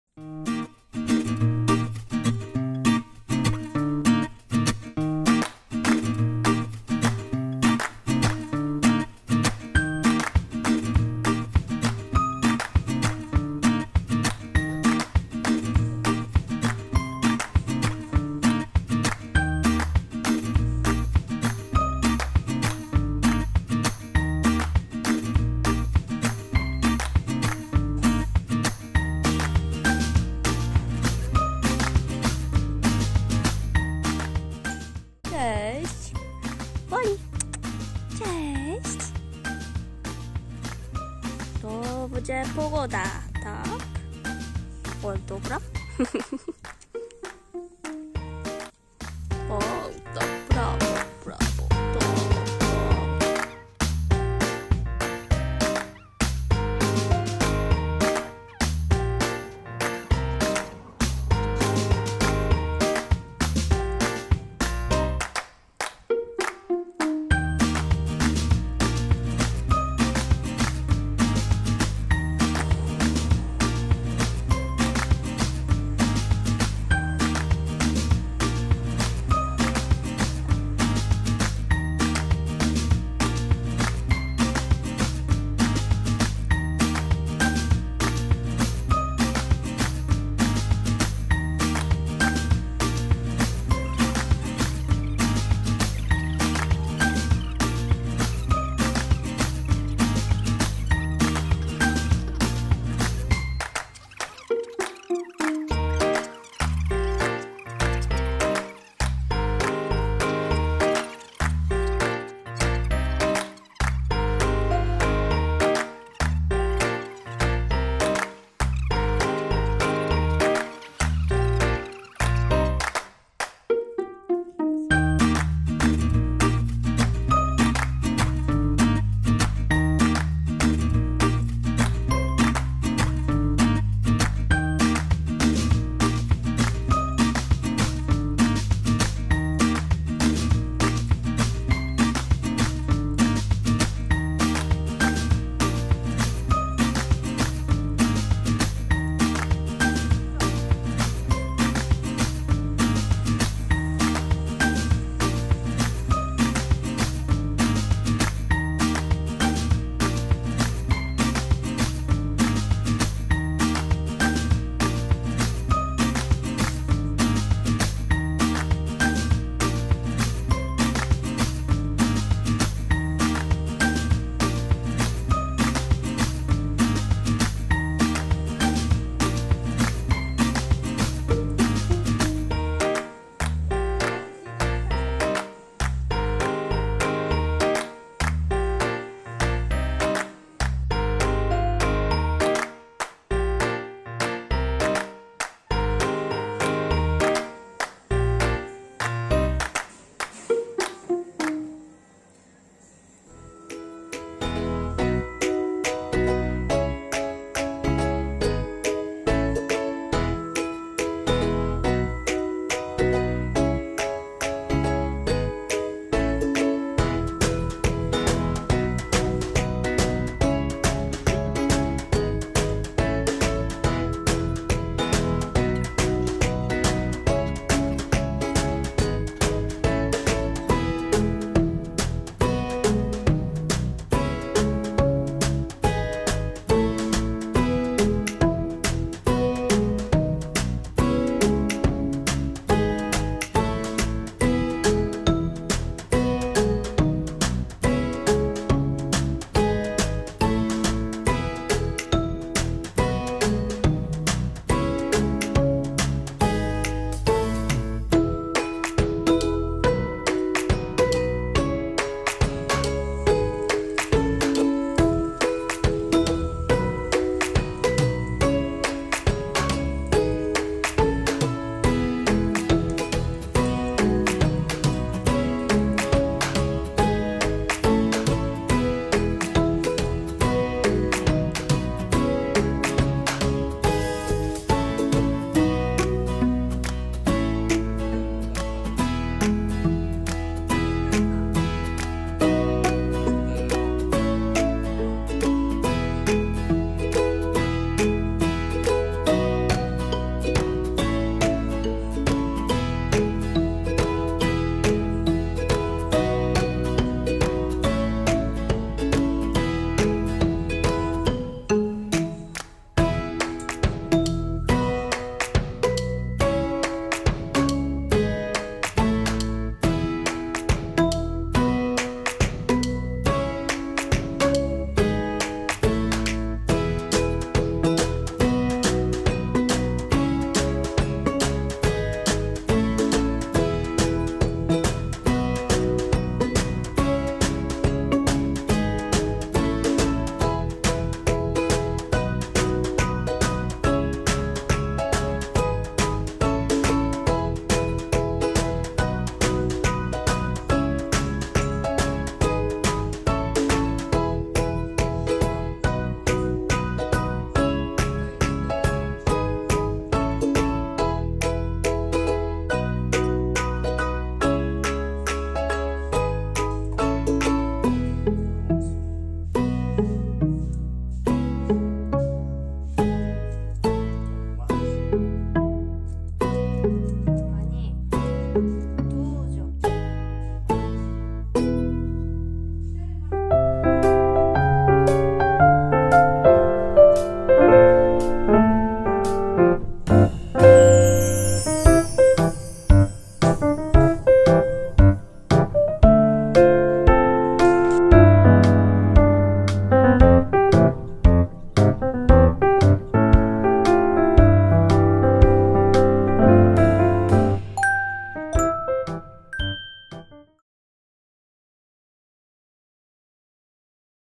[vlog#03] 🇵🇱 Pierwsze lato w Polsce! (feat. przygotowania ślubne)
Cześć! Jestem Lindsay
Dzisiejszy vlog pokazuje moje pierwsze lato w Polsce!
Głównie przebywam w domu, ale pokazuje też przygotowania do ślubu.